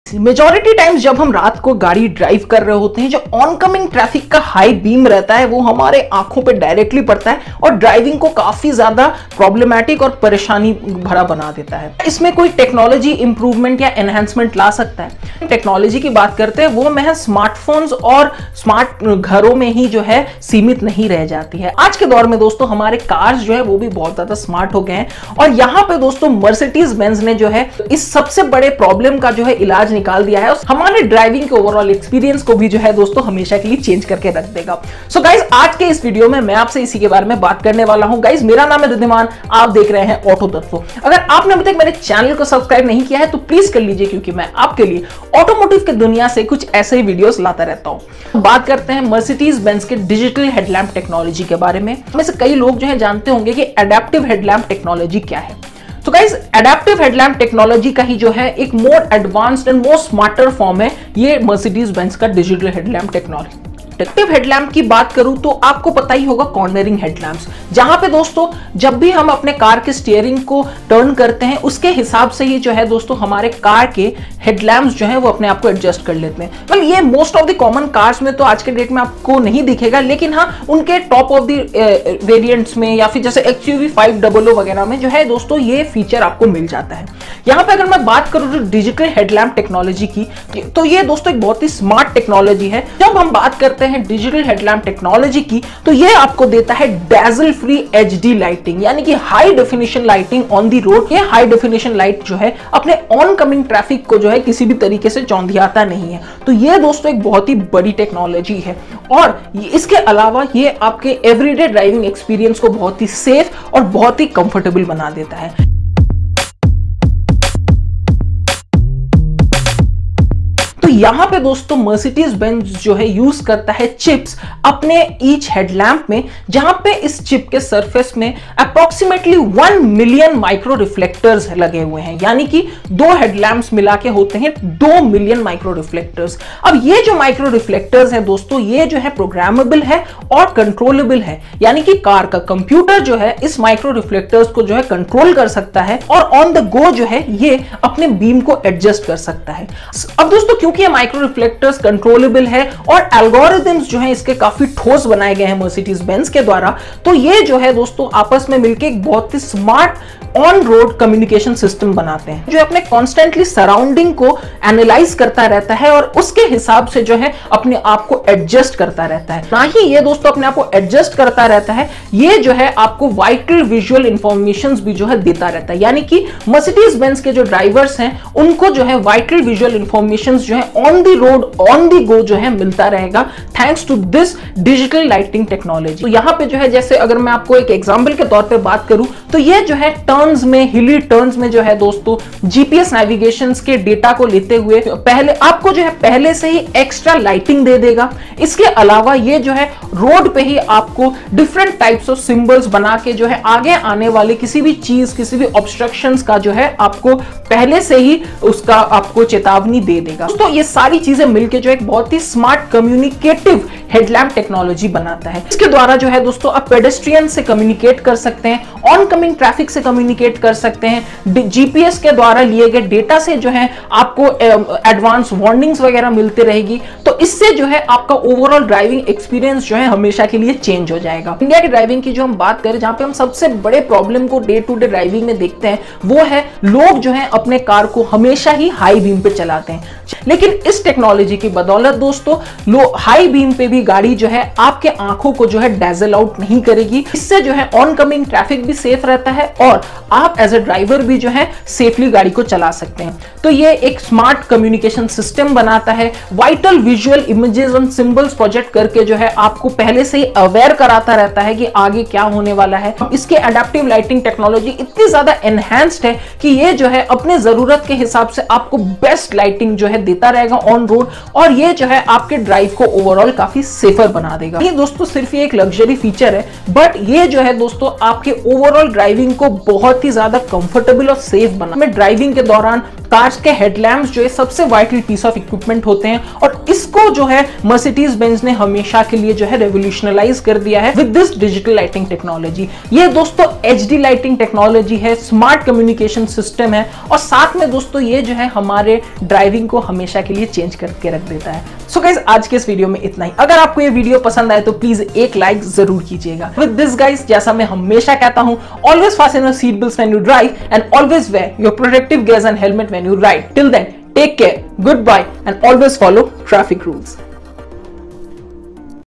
Mercedes Digital Light Technology : Best In Any Car Ever | Hindi | Auto Tatva
In this video I have shared a basic over view of the Mercedes Benz Digital Light technology. This technology help you get an amazing HD lighting and a lot more when it comes to cars. Currently seen in select Maybach, we all can hope to see this technology used in many other Mercedes Benz cars and maybe a lot of other cars. I hope you will like this video.

Links to my previous videos:

Mahindra Marazzo

Maruti Y1K (Maruti Zen)

Suzuki Jimny